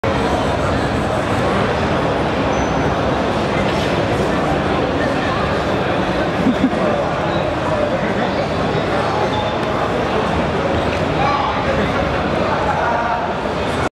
The Furry Is A Lie
Pen & Waffles fooling around with a Portal2-Cube on GamesCom 2012